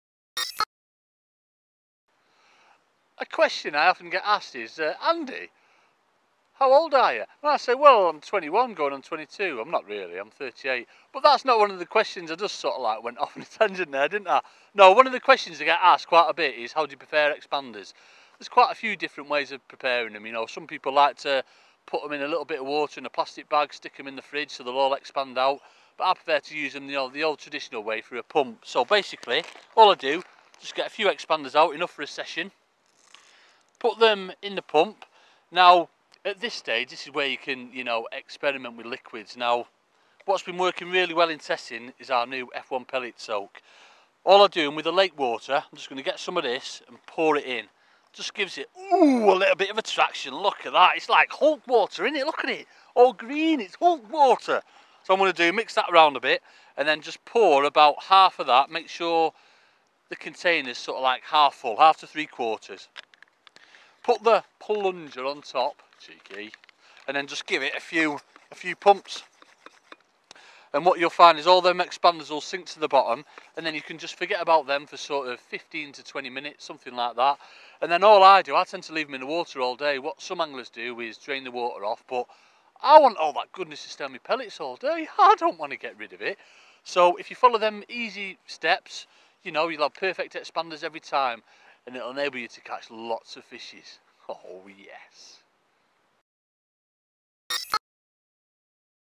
Andy May - How to Prepare Your Expander Pellets
A brief clip from match fishing ace Andy May on the best way of preparing your expanders for targeting carp and silverfish on a vareity of venues. Andy is fishing at Cudmore Fisheries in Staffordshire.